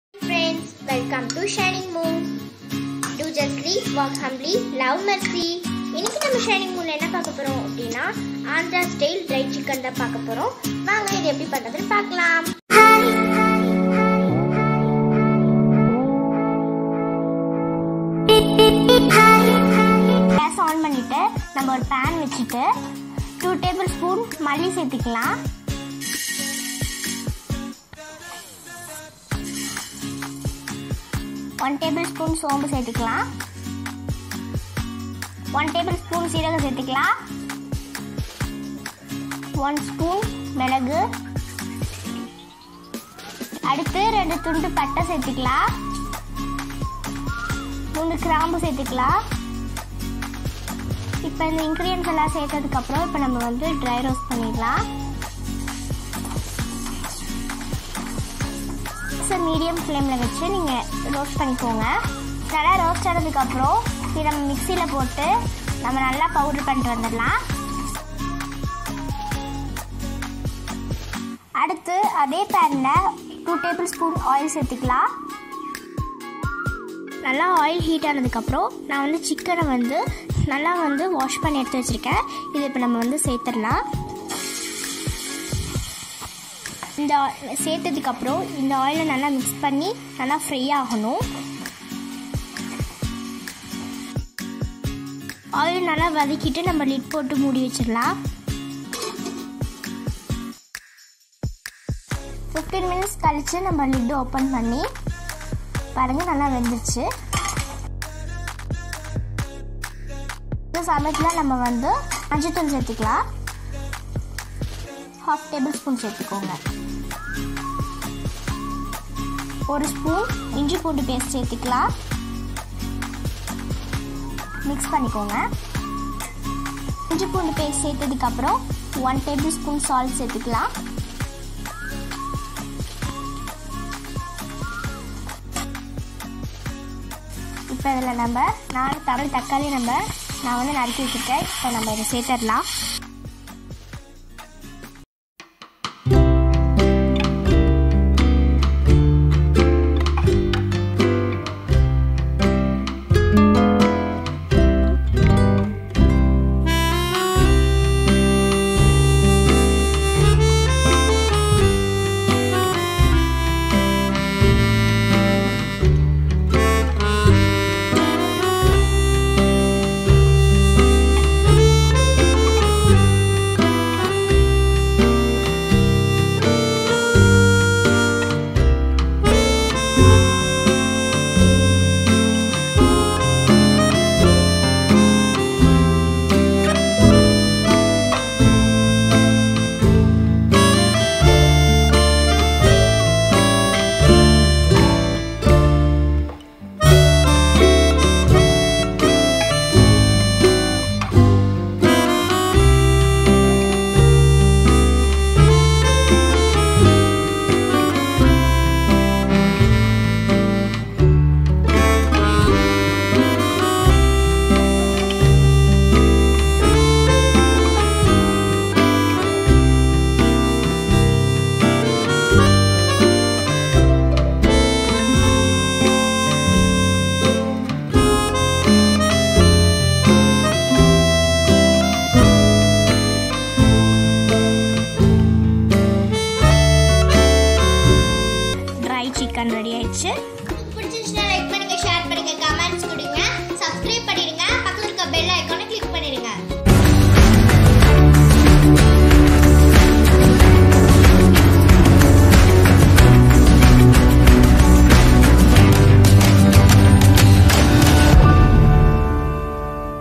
|| Andhra Style Dry Chicken in Tamil || tasty and spicy chicken recipe ||
hey guys,
        here is a andhra style chicken curry recipe in tamil try this and enjoy

do Justly walk Humbly love Mercy
this is " shining moon "